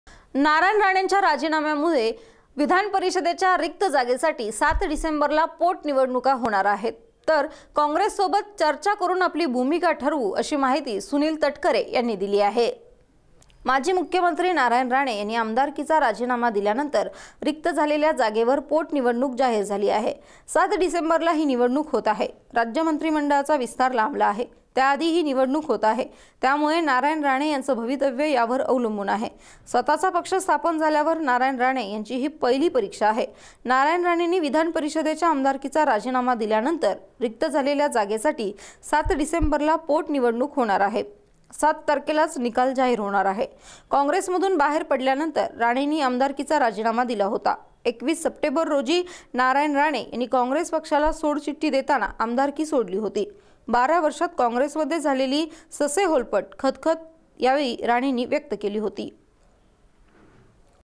नारायण राणेंच्या राजीनाम्यामुळे रिक्त विधानपरिषदेच्या जागेसाठी 7 डिसेंबरला पोटनिवडणूक
नारायण राणेंच्या राजीनाम्यामुळे रिक्त विधानपरिषदेच्या जागेसाठी 7 डिसेंबरला पोटनिवडणूक तर काँग्रेससोबत चर्चा करुन भूमिका ठरवू, सुनील तटकरेंची माहिती.@SBN मराठी.